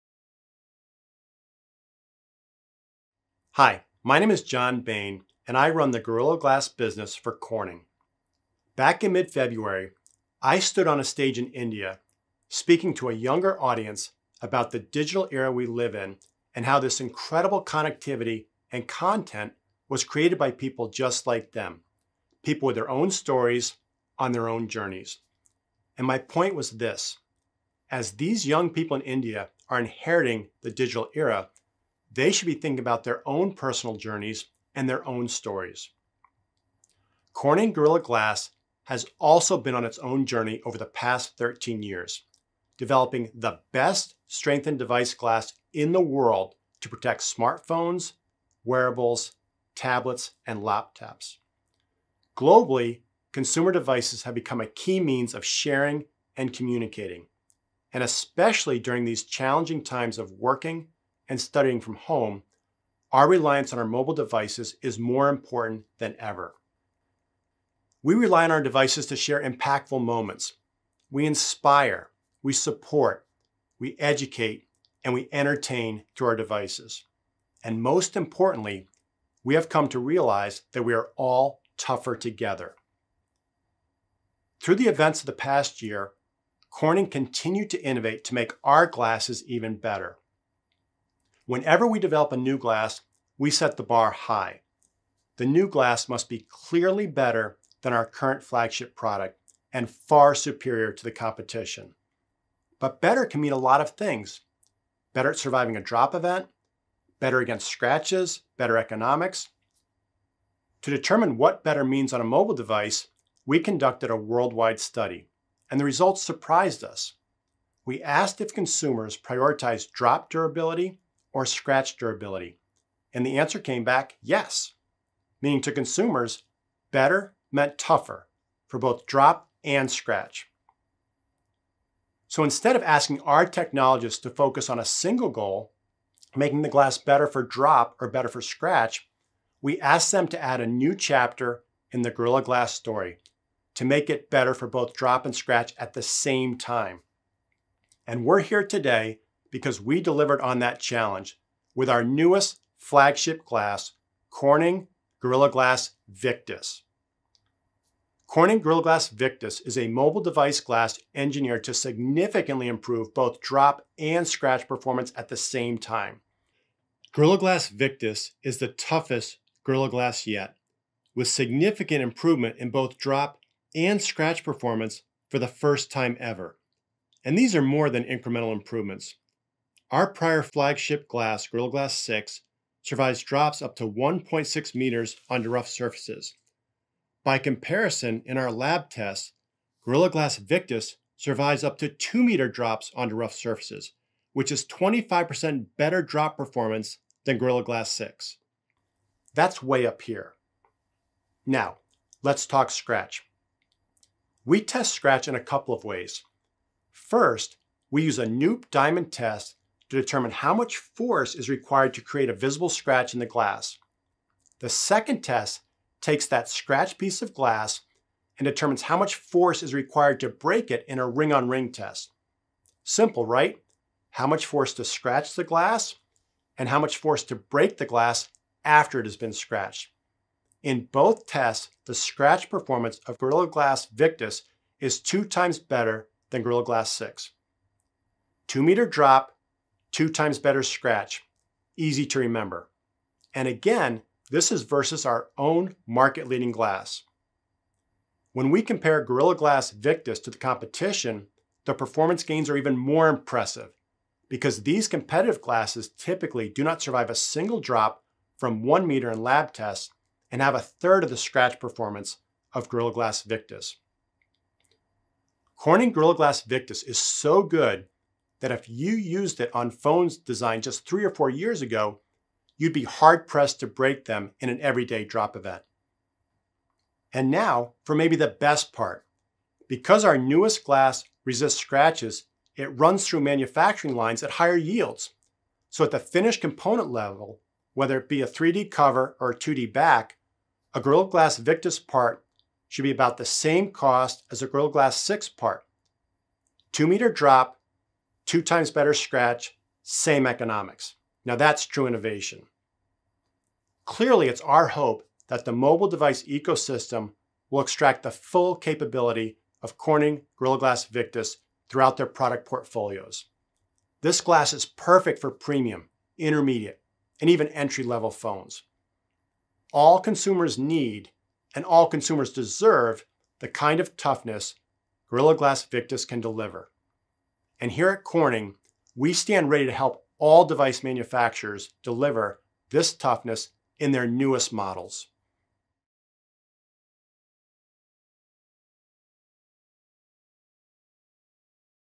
Corning Introduces Corning® Gorilla® Glass Victus™
Watch the launch-day introduction of our new glass by John Bayne, Senior Vice President and General Manager, Mobile Consumer Electronics.